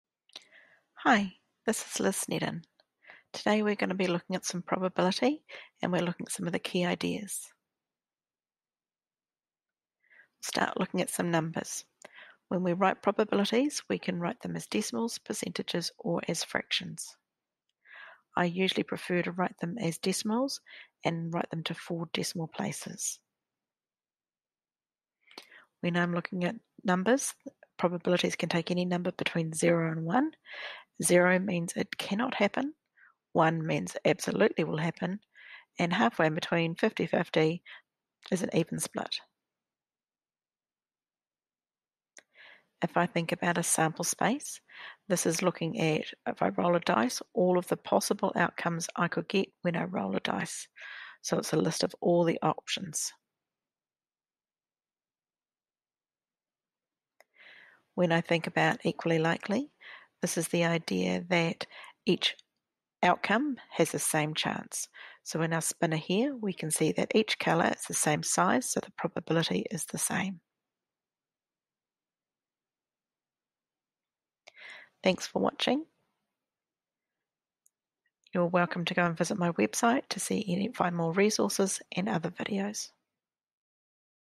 Probability Introduction
Introduction to key ideas: Values, Sample space, Outcomes.